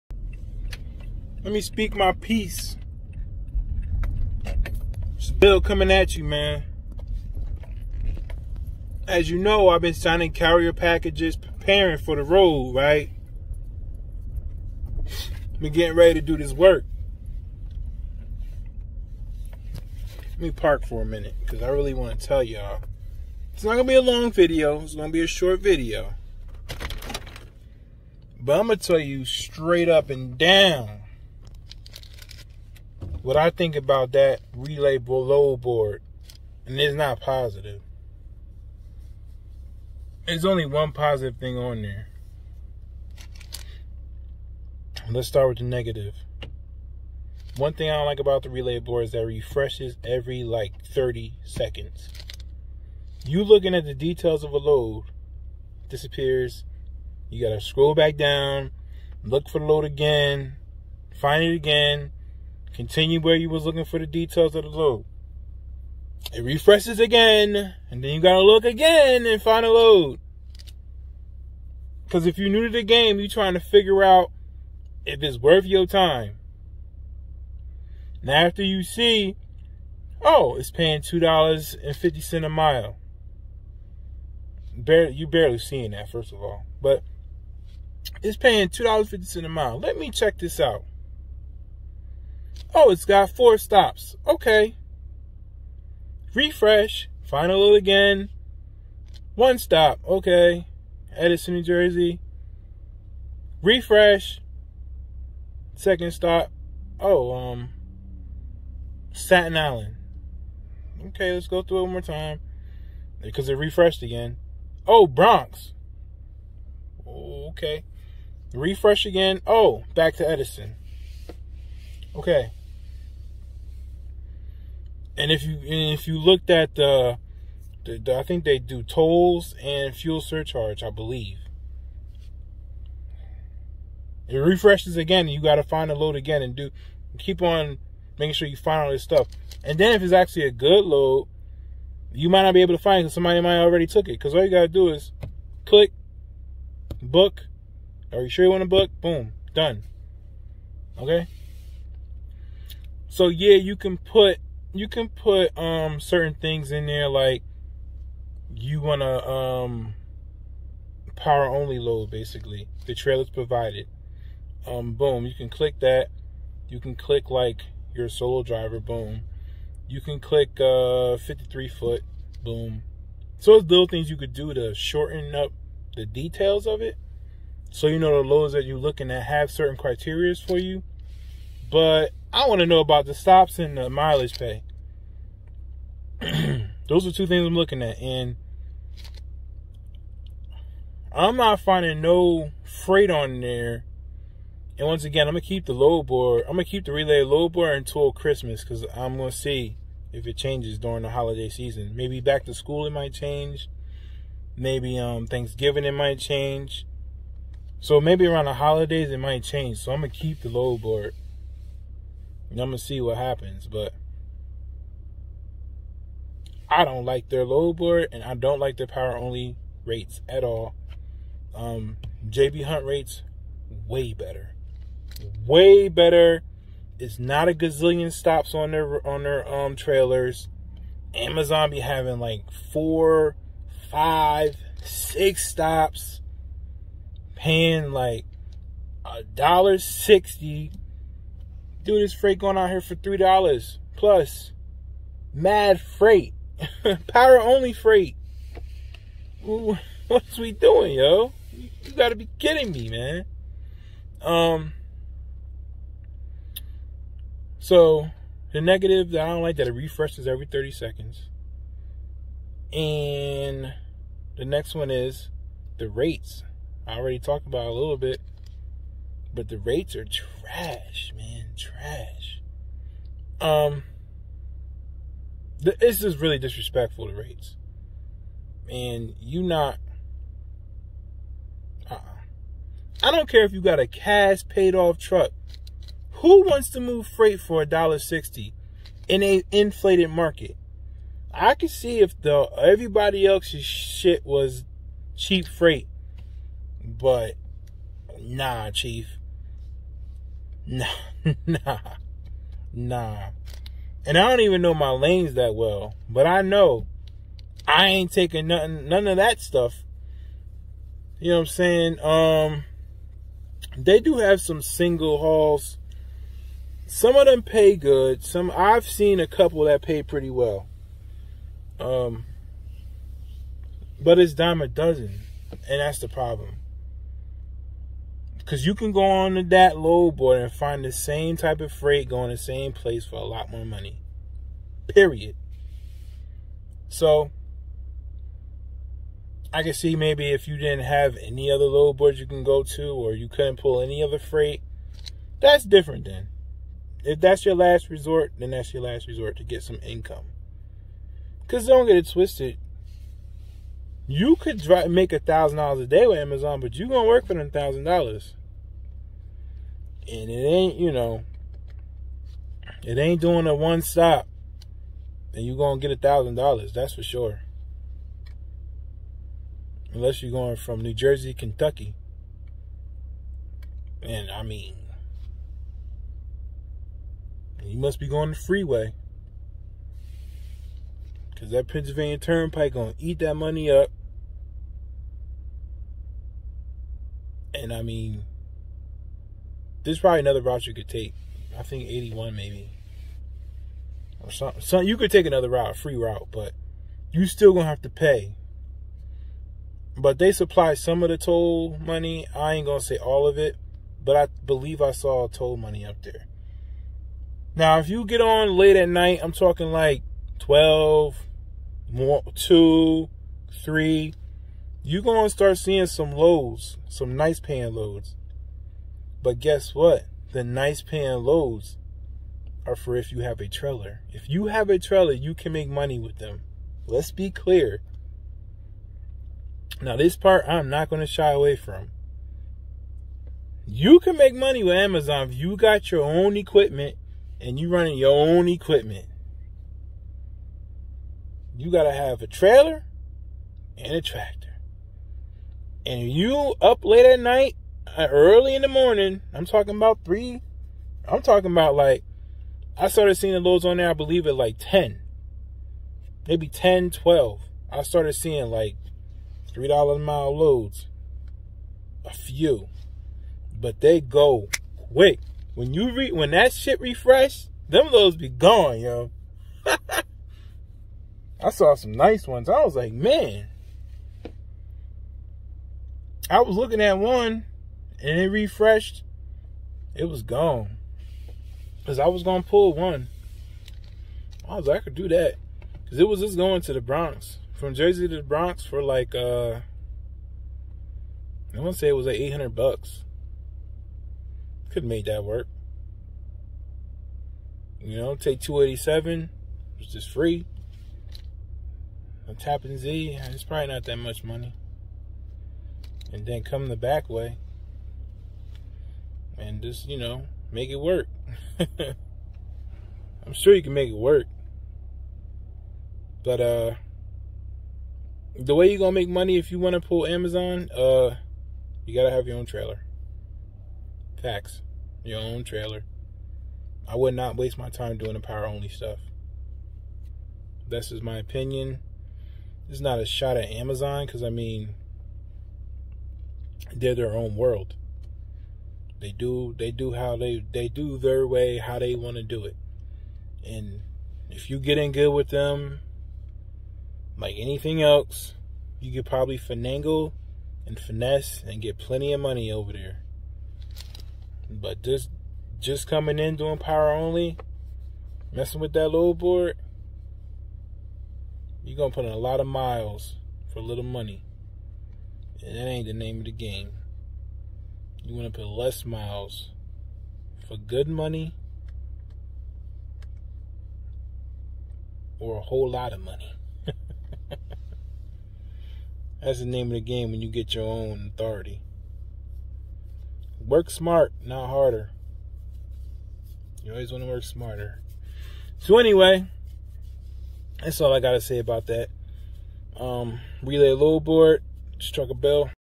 Amazon Relay Load Board My Opinion. ⁶⁰/⁴⁰
I'm not feel the power only side of Amazon...  If you have a trailer you can make some moves.